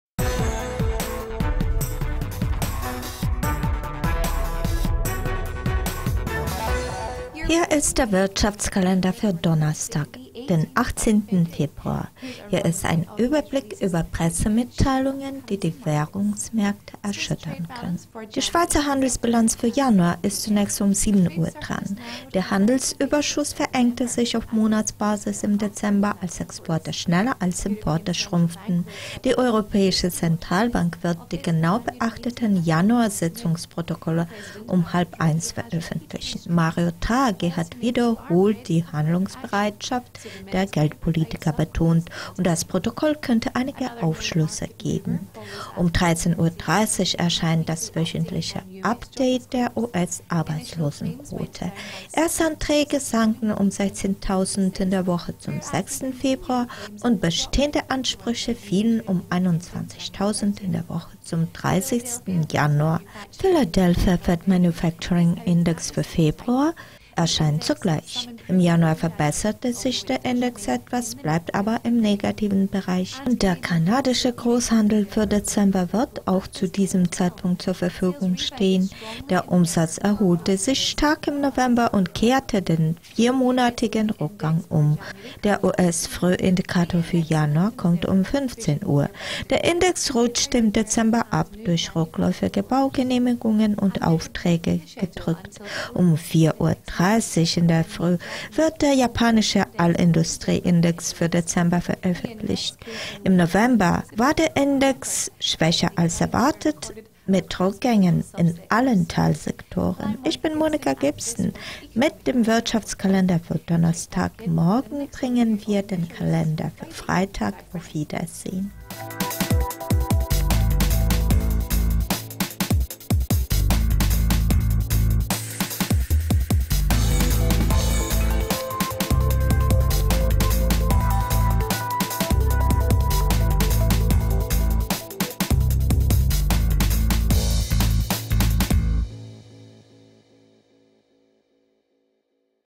Wirtschaftskalender
Monica Gibson listet wichtige Nachrichten auf, die für  Devisenhändler am Donnerstag, 18. Februar von Interesse sein könnten.